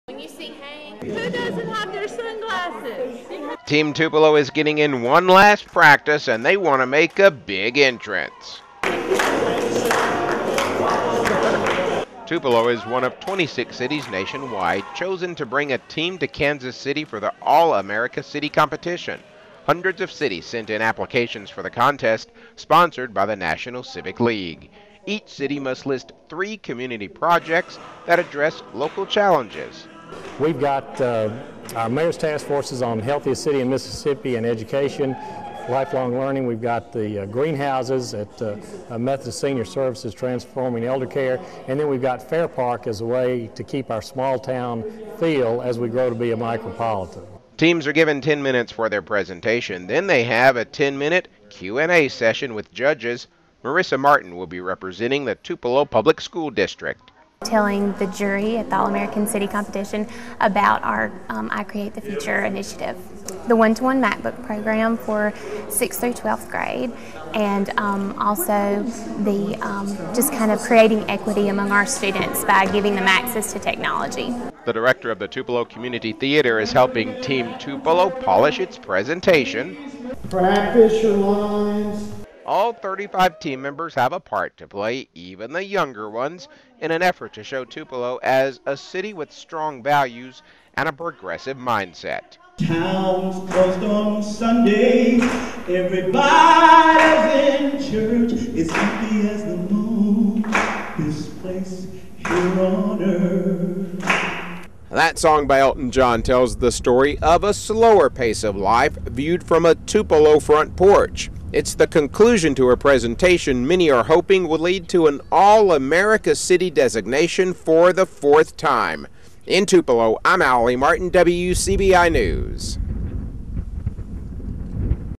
wed all america city competition pkg.mpg
Story from WCBI's Allie Martin on Tupelo's quest to be named an "All America City" for the fourth time.  This story takes the viewer to the final practice before "Team Tupelo" headed off to Kansas City, Missouri for the competition.